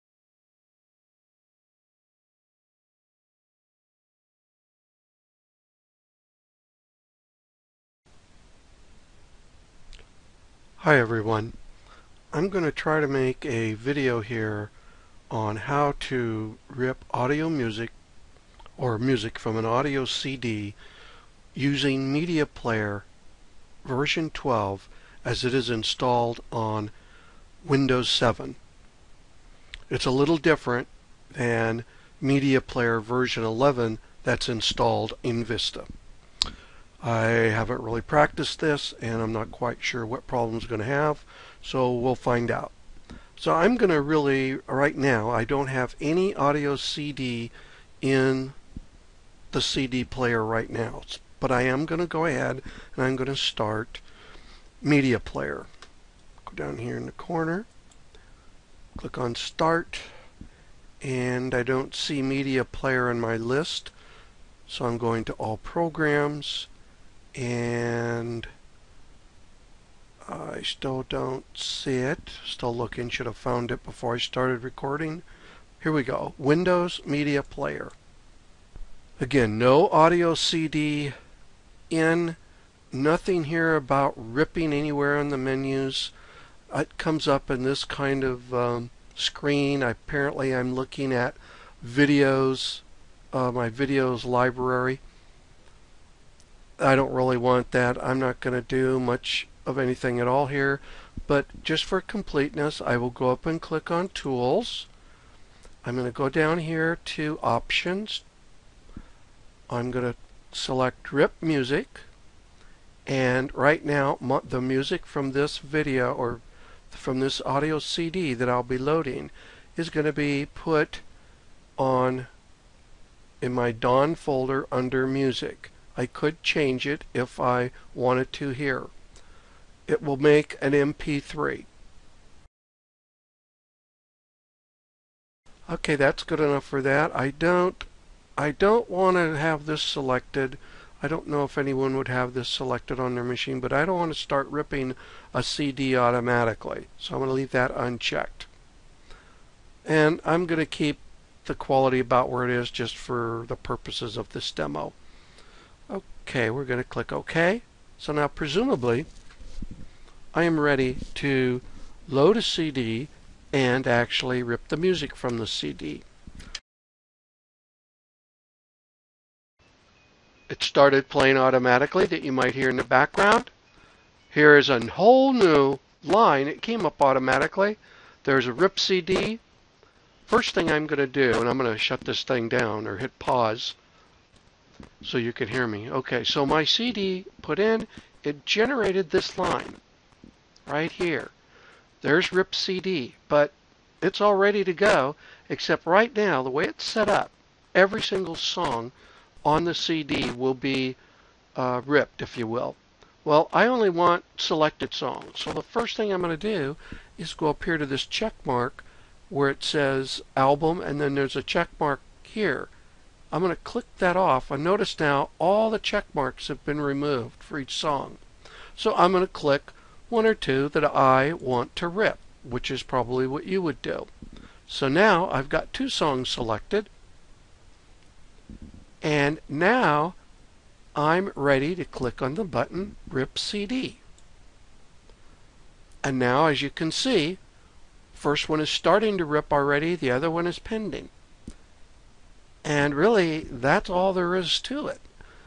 Rip Music Using Win 7 Media Player
In this demonstration, we will rip selected songs from an audio CD using Windows Media Player 12. It is this version that is installed on Windows 7.